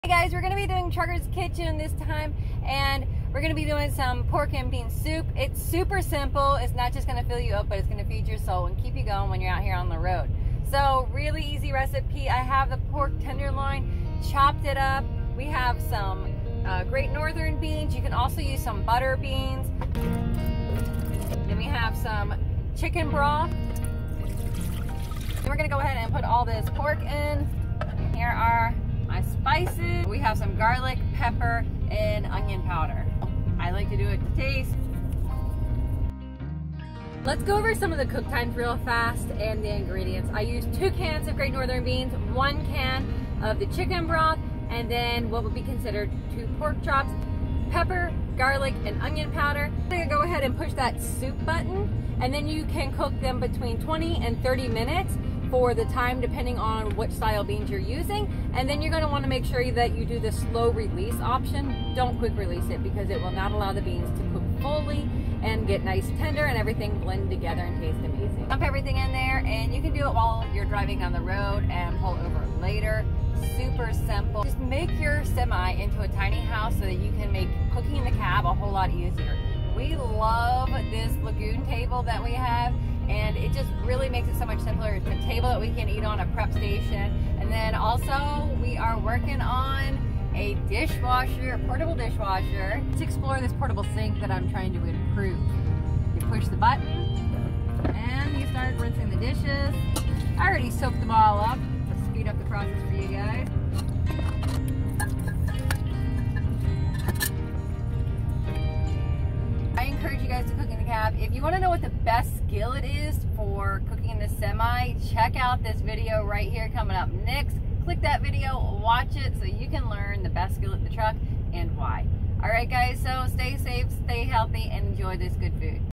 Pork & Bean!! How to COOK IN A SEMI
Soem meals can be aimple, filling ann feed the soul!! The pork and bean recipe is just that.

2 cans Great Northern Beans
1 can chicken brooth
2 pork chops
Paper, garlic powder and onion powder to taste

Also check out the lugon table and the best skillet for cooking in the cab.

Be careful, stay safe and enjoy the adventures.